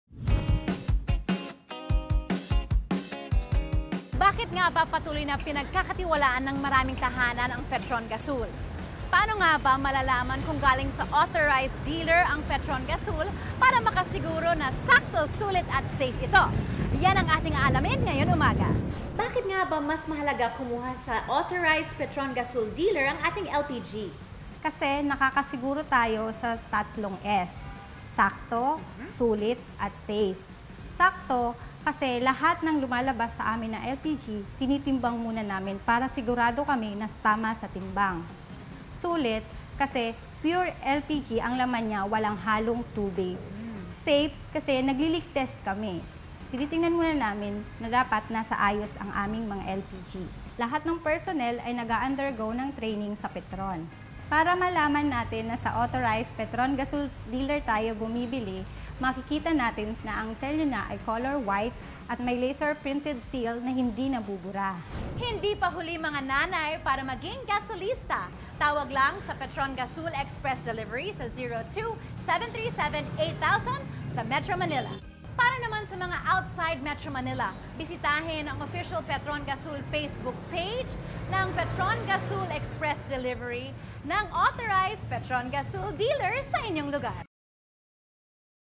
How to know if your LPG is from an authorized dealer
How can you be sure that the LPG product you buy for your home is from an authorized dealer? With Petron Gasul, you can be assured that the product you get has these important components: sakto, sulit, and safe. Find out more from Regine Tolentino in this video.